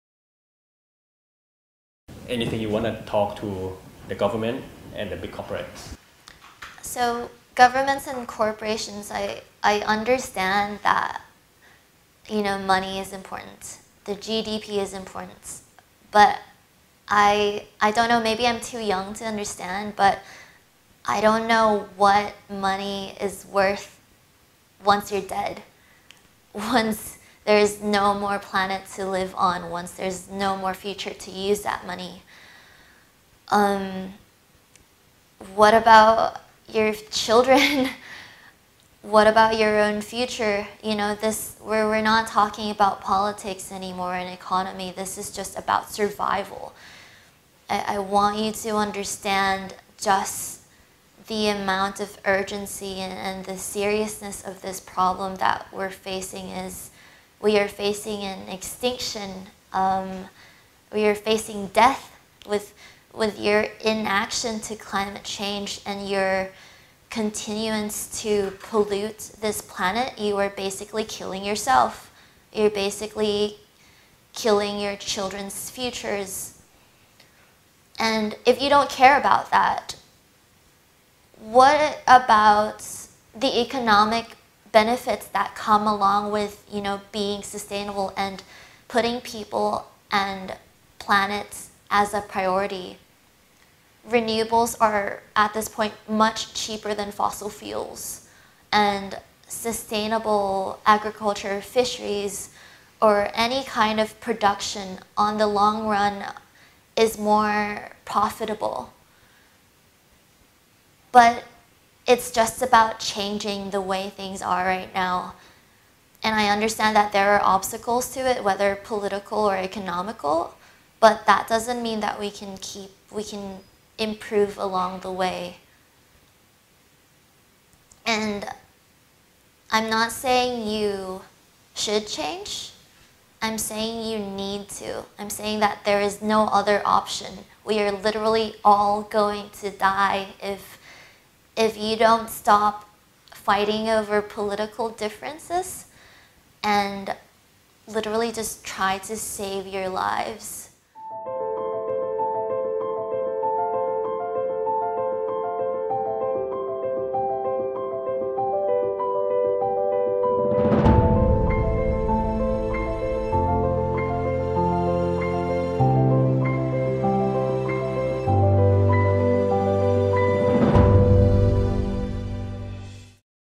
ไม่อยากตาย ต้องเร่งแก้ปัญหาโลกร้อน หลิง นันทิชา Climate Strike - Workpoint News
เกาะติดทุกข่าวที่น่าสนใจ กับข่าวเวิร์คพอยท์
การเมือง สังคม เศรษฐกิจ ต่างประเทศ นโยบายสาธารณะ สุขภาพ คมนาคม

Podcast by Workpoint News